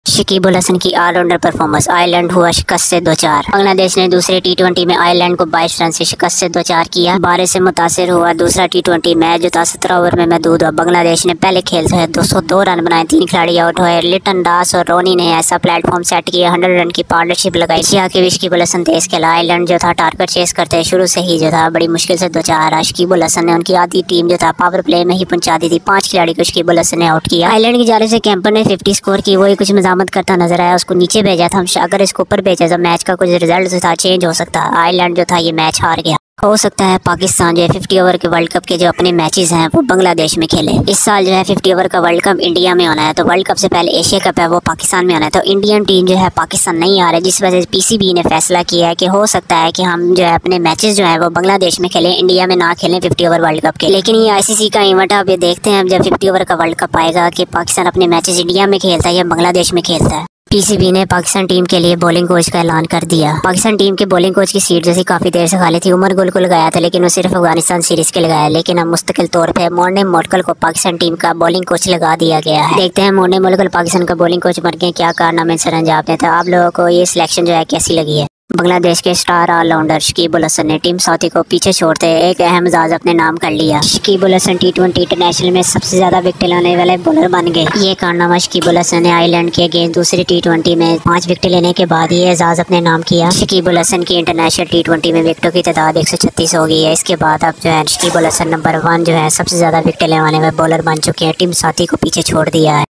Top 4 cricket news|Banvsireland 2nd t20|Pakistan play their matches in Bangladesh|shakib Al Hassan
in this video you can watch top 4 cricket news today:-
1. Bangladesh vs Ireland 2ns T20 highlights
2. Pakistan team could play their world cup 2023 in Bangladesh
3. shakib Al Hassan most wickets in t20 international
4. morne Morkel Appointed Pakistan team bowling coach